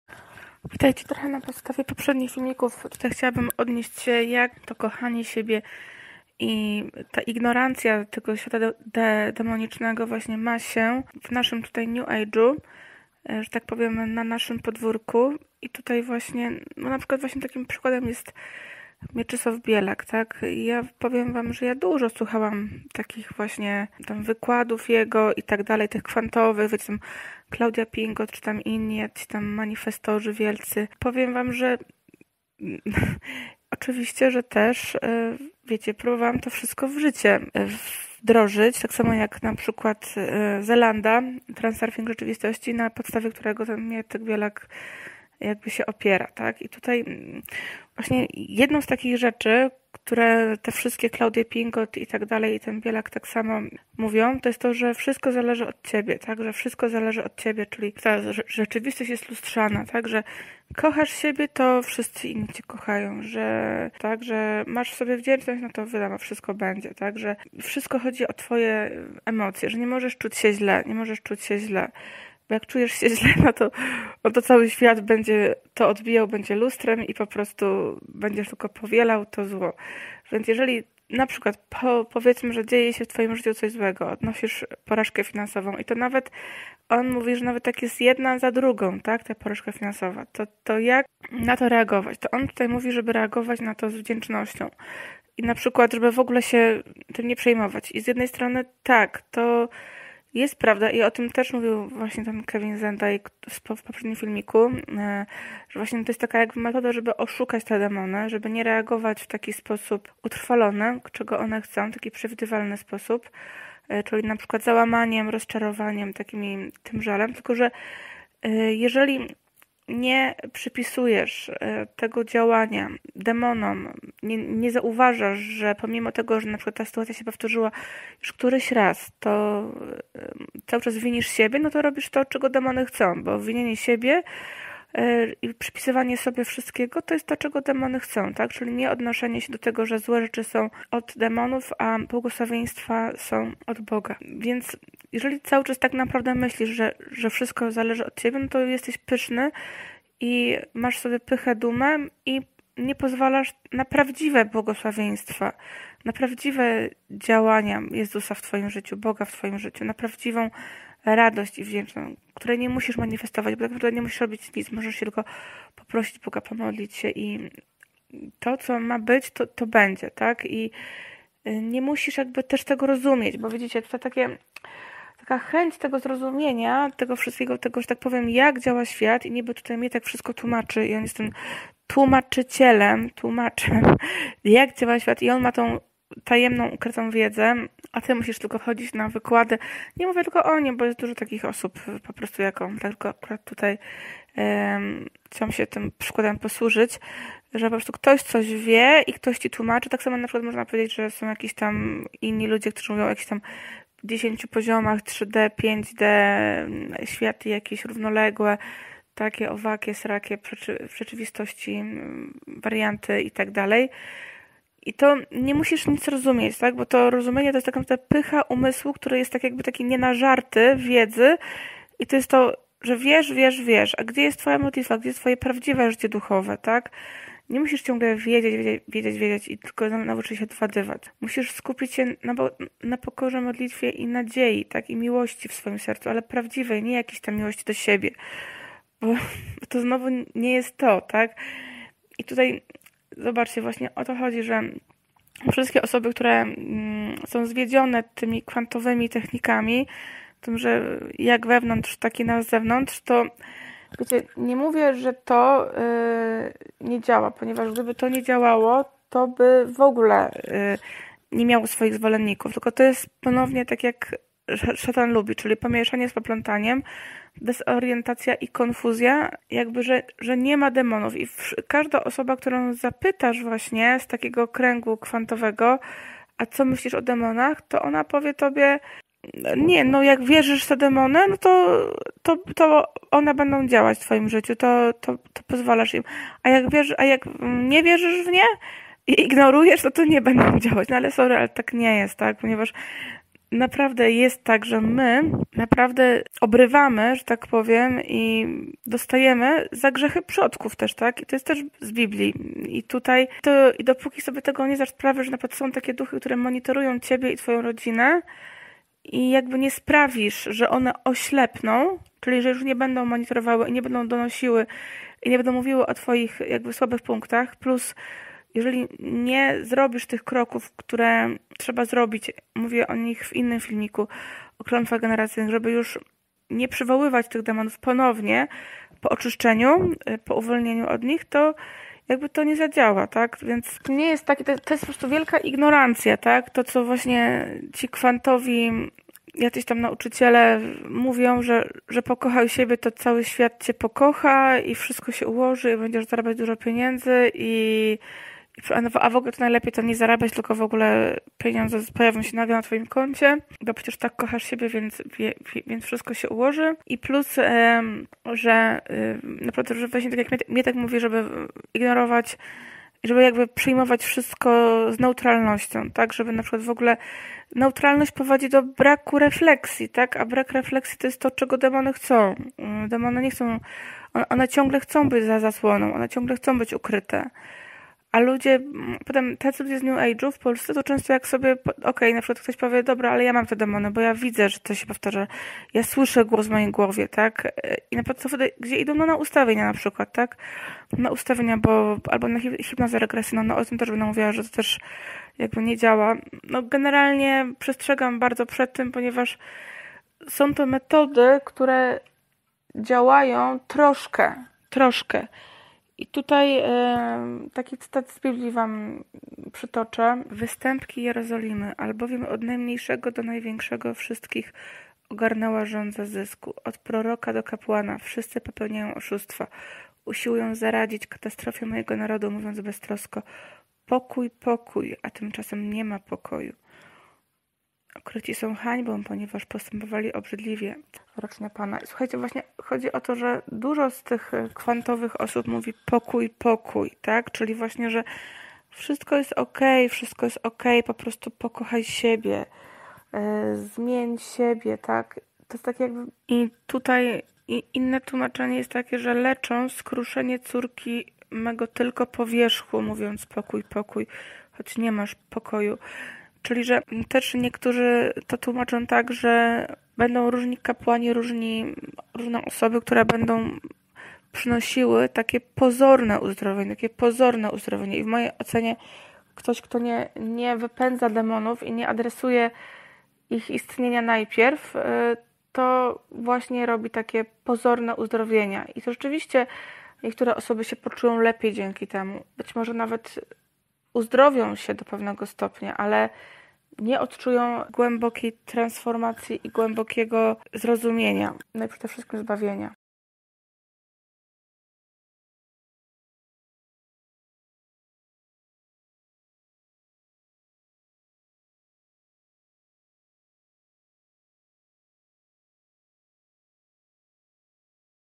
Czy świat naprawde jest lustrem? Nienażarty umysł Tłumaczyciele świata Mietek Bielak Klaudia Pingot
Czy świat naprawde jest lustrem? Kwantowy i fraktalny? Nienażarty wiedzy umysł "Tłumaczyciele świata moje swiadectwo